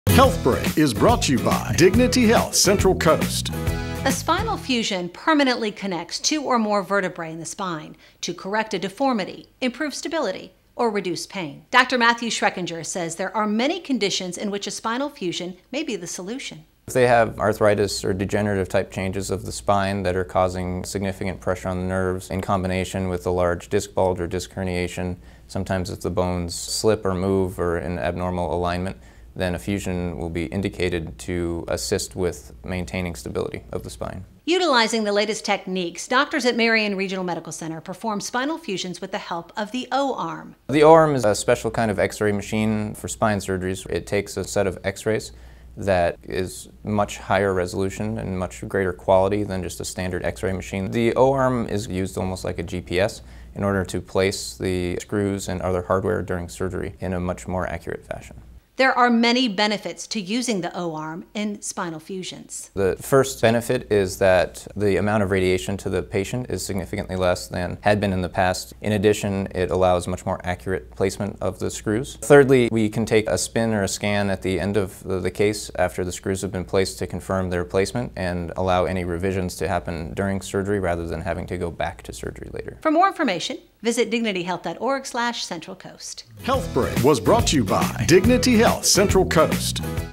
HealthBreak - Spinal Fusion
Matthew Schreckinger, MD, Neurosurgeon, Santa Maria Specialty Health Center, discusses spinal fusion. See Dr. Schreckinger's Profile